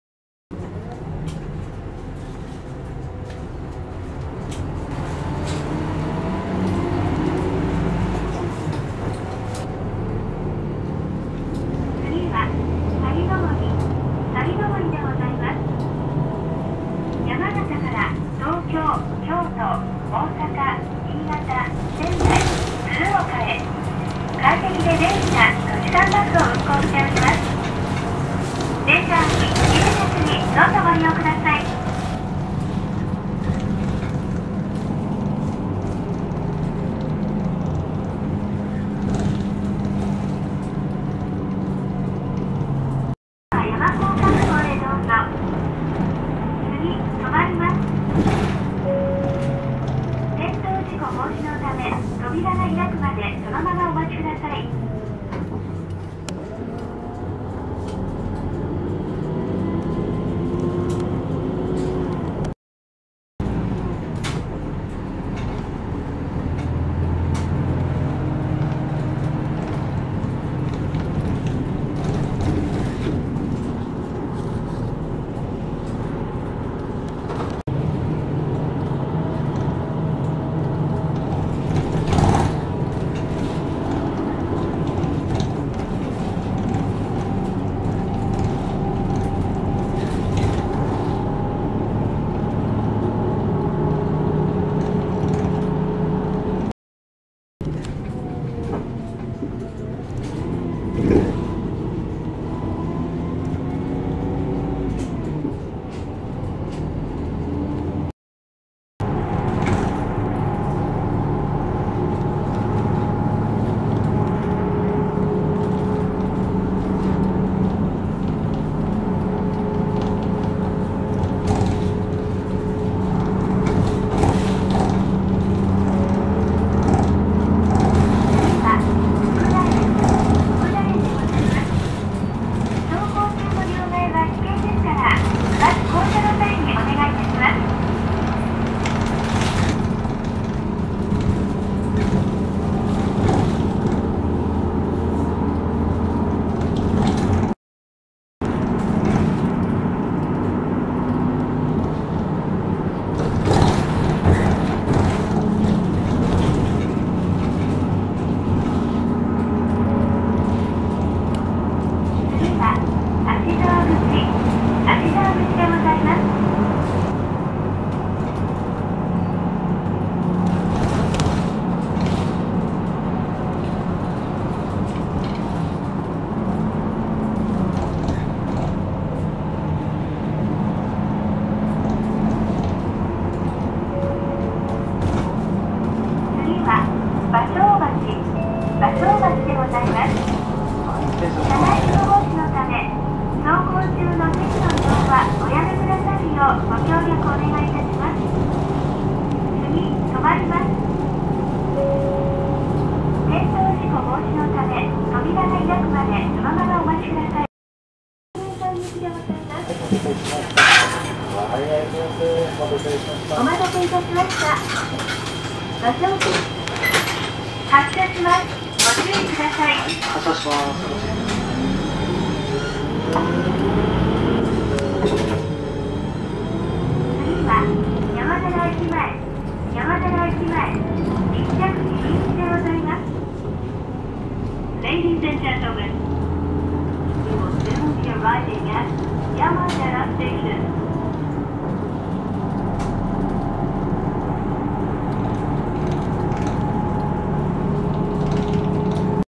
山寺へ 山形県（2018年青春18切符 夏の旅 19/62）
山形城から

山形駅に戻って

JR仙山線で山寺に行くつもりが

山形駅前のバス停に

ちょうど山寺行きのバスが

入ってきたので

おもわず飛び乗った。

JR仙山線快速では

山形駅から山寺は

５駅目で約２０分だが

バスと鉄道では

時間も値段も違うのでは、、

今さら下車しても

戻りのバスの時間も

地理感も分らないので

そのまま山寺まで

行くことにしました。